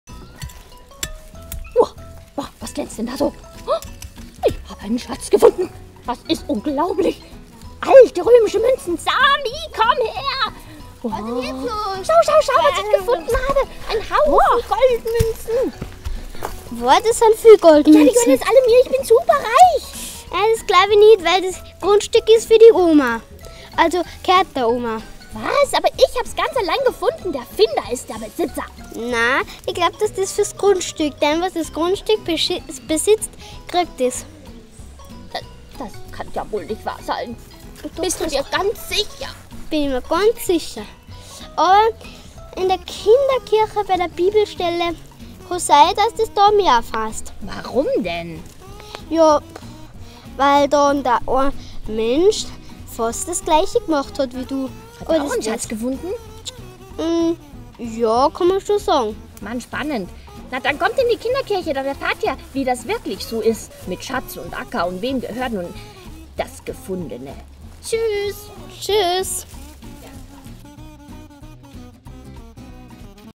Xaver und der Schatz im Acker
Wem gehört der Schatz? Dem Finder, oder dem Besitzer des Grundstückes, auf dem der Schatz gefunden wurde?